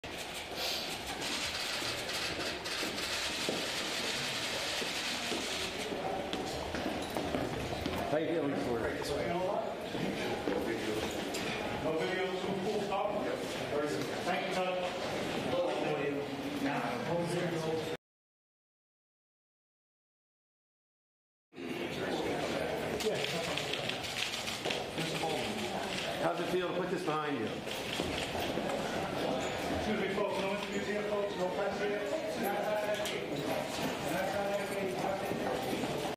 Alec Baldwin pleads guilty to harassing motorist
(23 Jan 2019) Alec Baldwin has pleaded guilty to harassing a man during a dispute over a parking spot last fall. The 60-year-old actor appeared in a New York City courtroom Wednesday.
He agreed to complete a one-day anger management class to resolve the criminal case. The charge is a violation, the lowest level of offense. If he completes the class, the case record will be sealed.
Baldwin was accused of trying to punch another driver during a Nov. 2 argument over a parking spot in front of his Manhattan apartment building.
The former "30 Rock" star's lawyer had said Baldwin would be vindicated by video evidence.
Baldwin only spoke a few words during the court hearing, mostly answering short questions from the judge. He wouldn't comment afterward.